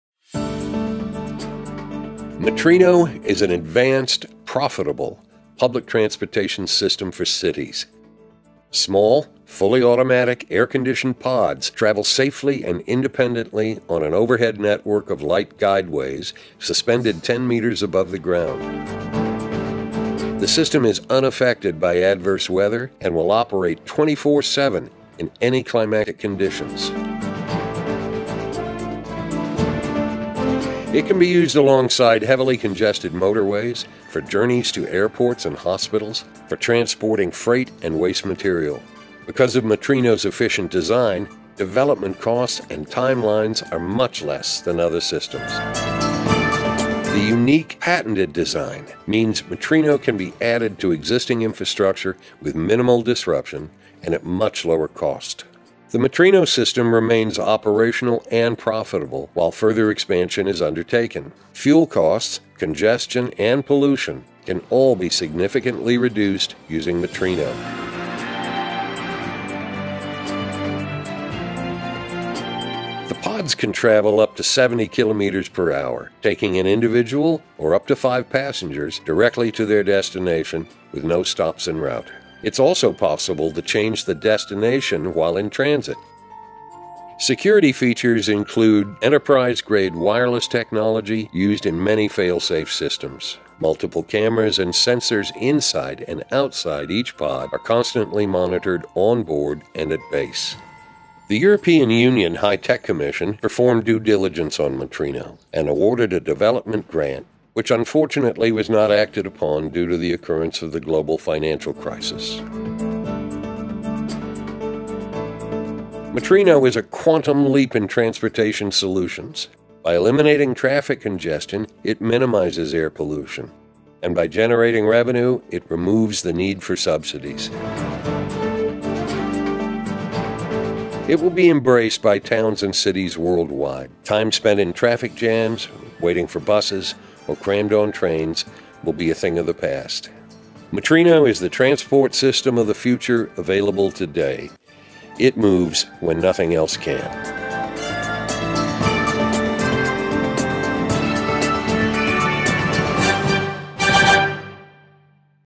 Metrino PRT 2TS
Public city transportation of the future available today.
"METRINO moves when nothing else can".
Best of all PRT designs.
Spread the word if you' want to have it in your city.

The world’s best 21st century urban transit system.

Technically the most advanced, Metrino-PRT has the least interference to daily functioning of the city during construction.

A solution which reduces traffic congestion and automotive pollution while raising the quality of urban life.

This state-of-the-art public transport will save authorities millions of dollars in development costs and annual infrastructure. Metrino-PRT is the most economically viable system.

Metrino-PRT provides unrivaled capacity, speed, safety and comfort for passengers.